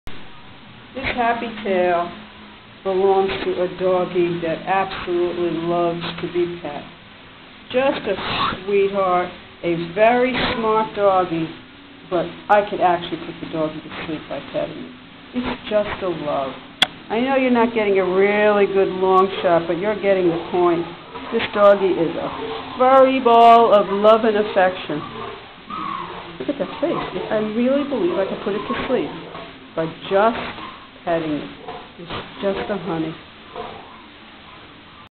Meet Banjo a Beagle currently available for adoption at Petango.com! 4/10/2013 3:04:58 PM
Banjo is a super sweet doggie. His tail wags so much that you can even see it wag in still photos (see photos 2 & 3). I wanted him to look great for his "close up" so I spent time cleaning his face. He was very happy to oblige. He just loves to be pet. This affectionate doggie will tug at your heart strings and bring joy and companionship into your life.